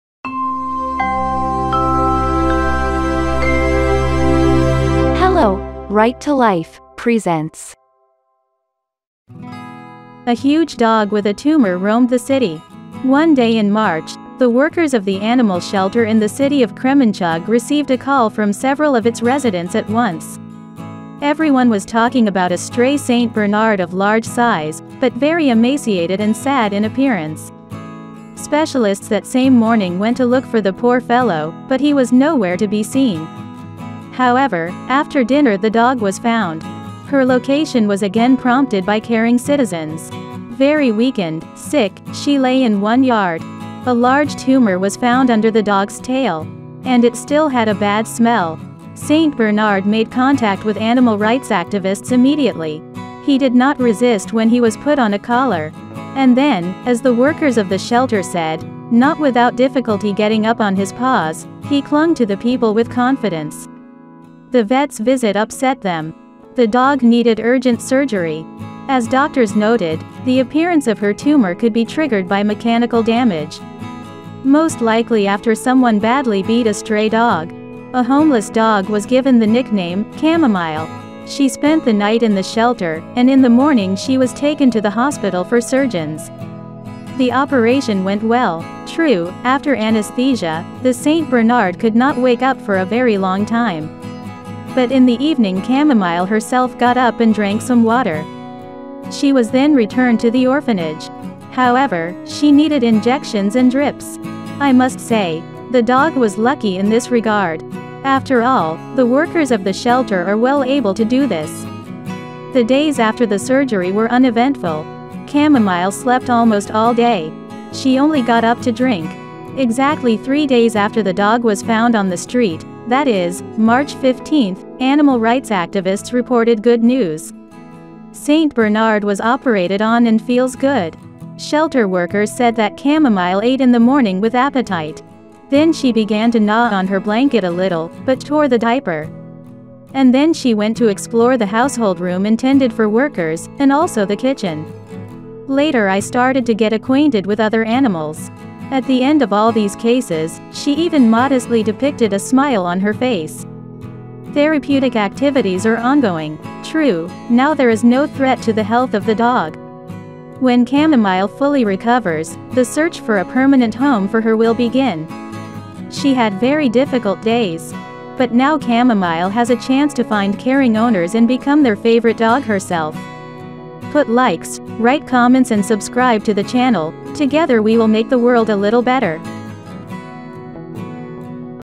A huge dog with a tumor roamed the city.
Right to Life - A huge dog with a tumor roamed the city.

One March day, several of its residents called the workers of the animal shelter in the city of Kremenchug at once. Everyone was talking about a stray St. Bernard of large size, but very emaciated and sad in appearance.